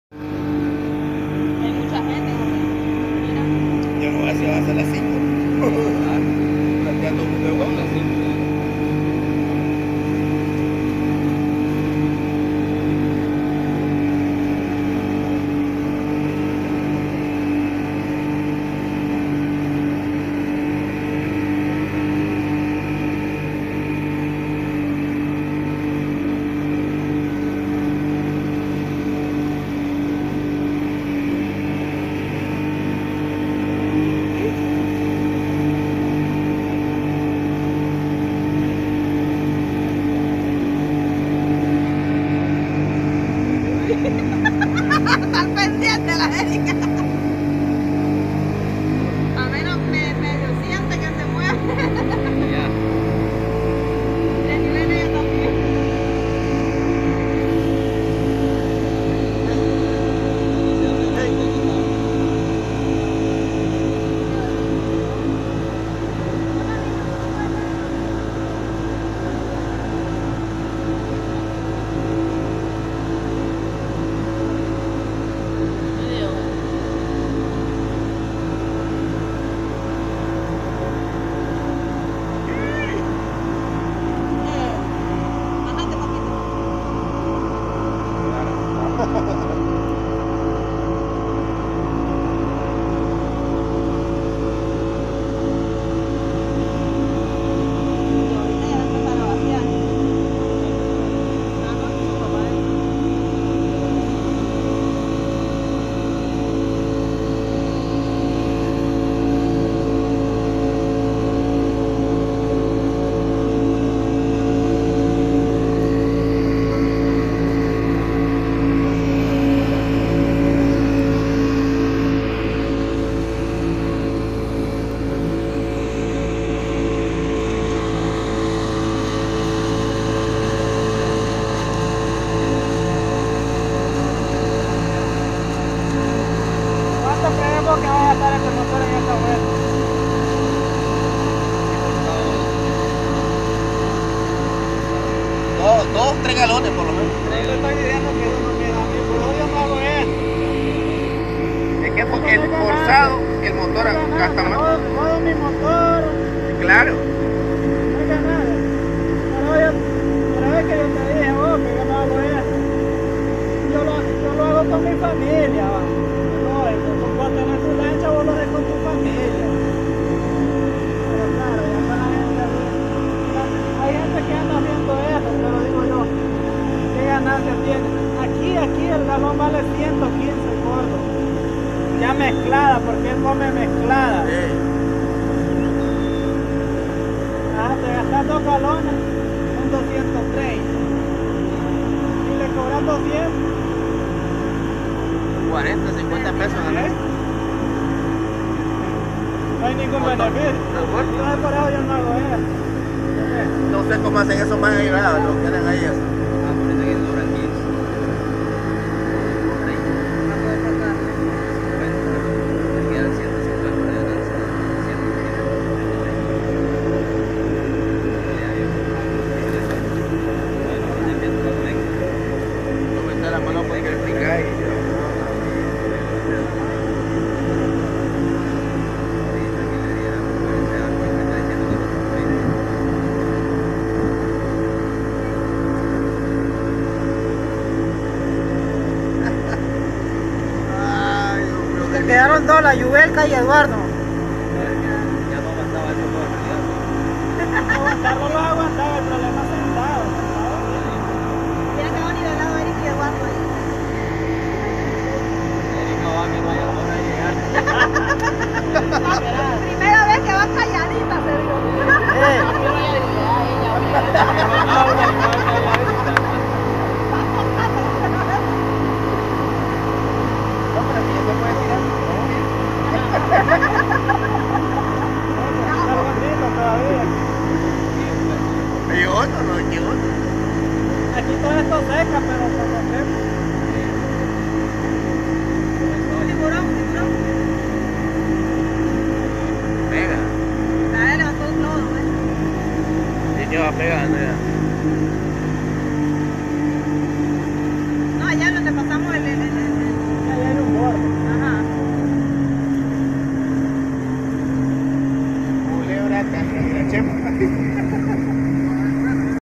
Nicaragua. Playas ( padre ramos)
En lo que fue la semana santa del 2016 vivimos esta peligrosa aventura,  que varios nos sumimos en pánico por lo arriesgado que fue. Pero en una aventura hay riesgos y eso la hace una gran aventura.